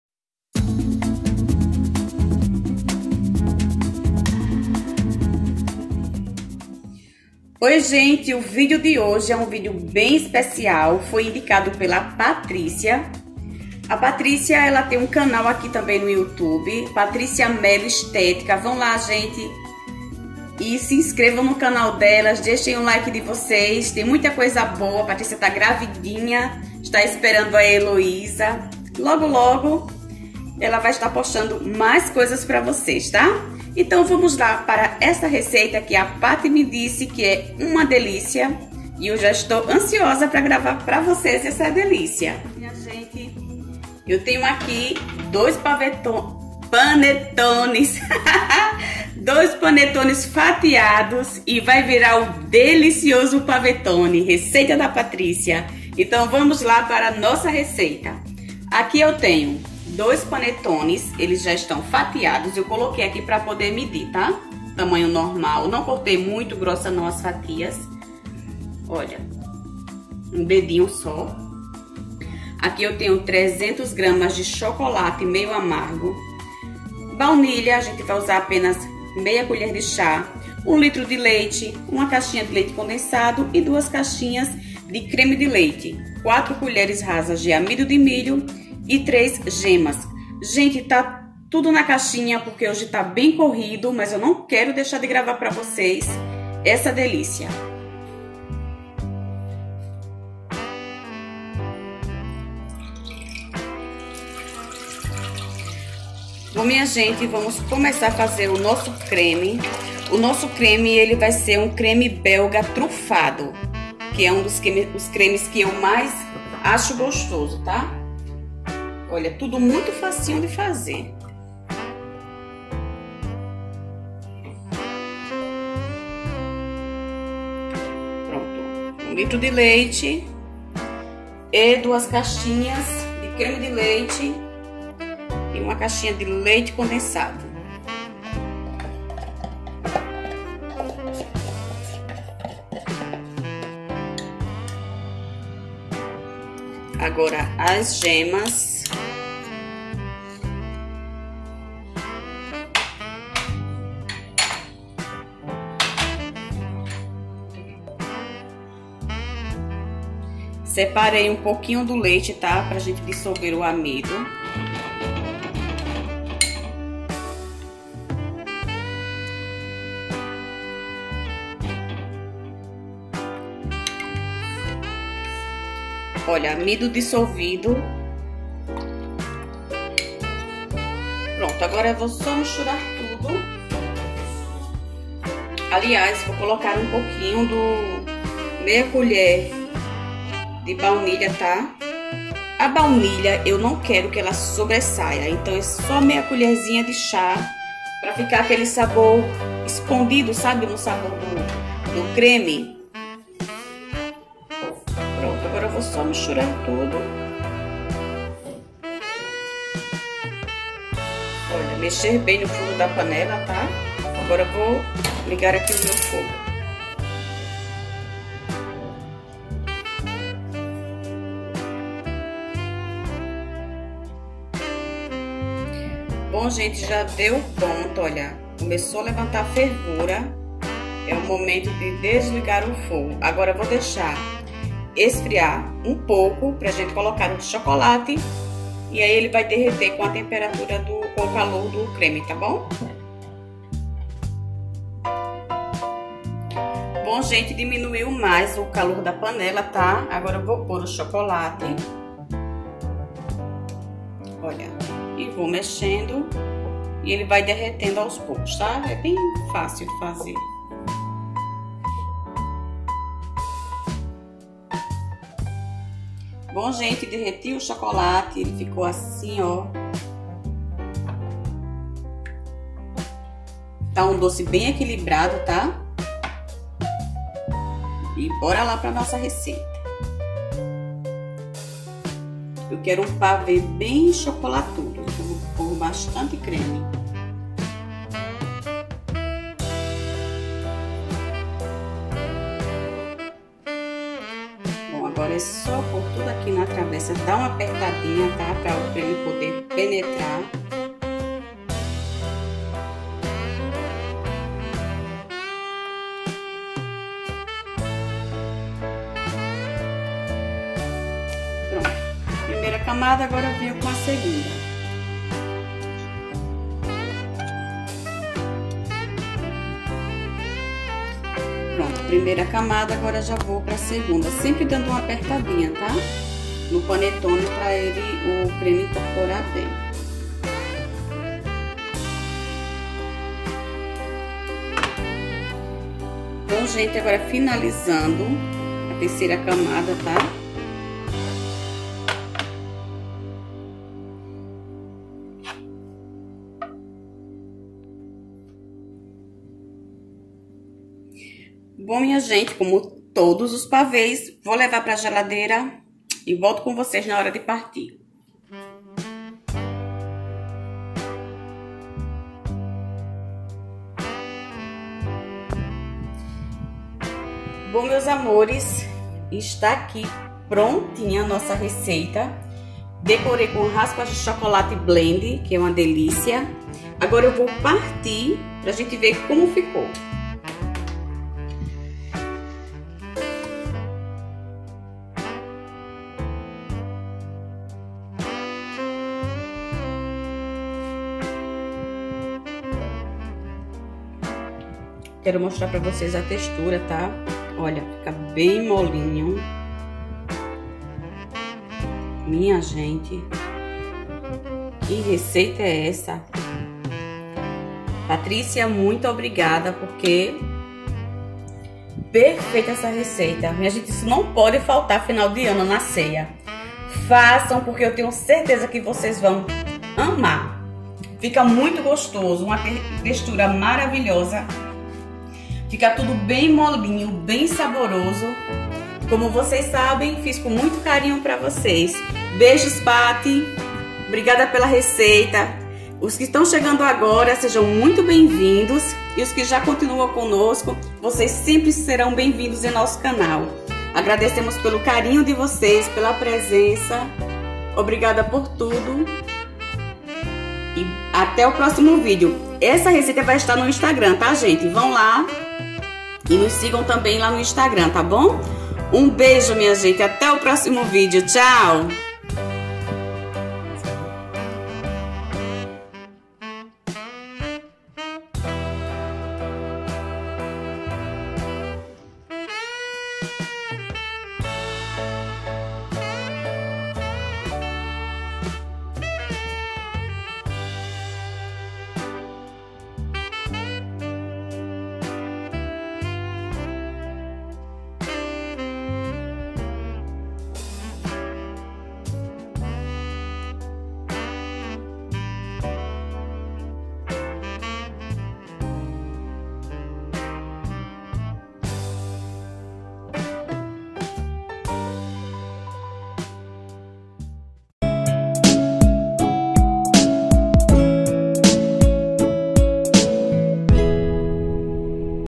PAVETONE CHOCOLATUDO - PASSO A PASSO
PAVÊ DE PANETONE COM RASPAS DE CHOCOLATE

INGREDIENTES:

2 PANETONES DA BAUDUCCO OU DE SUA PREFERENCIA
1 CX DE LEITE CONDENSADO
2 CX DE CREME DE LEITE
1 LITRO DE LEITE
3 GEMAS DE OVO
300 G DE CHOCOLATE MEIO AMARGO
½ COLHER DE CHÁ DE ESSÊNCIA DE BALNILHA
3 COLHERES RASA DE AMIDO DE MILHO